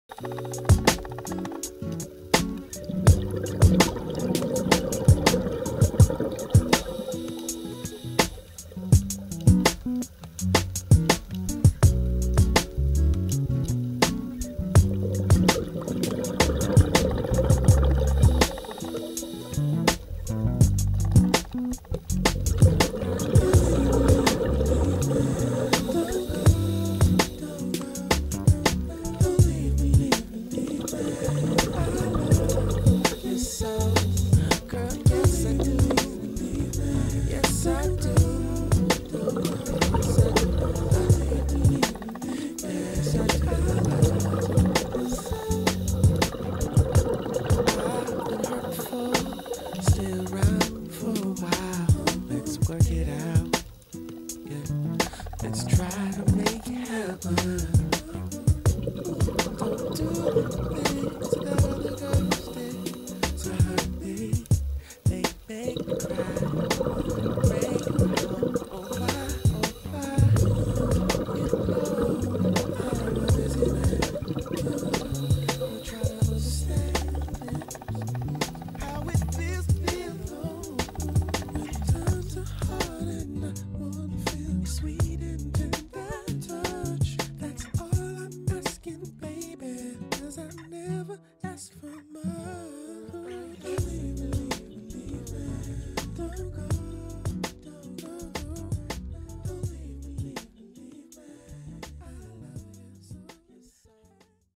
Dive Mania No 161
良い海況が続き「海」青いです。
本日、ハタ類が目に付きました！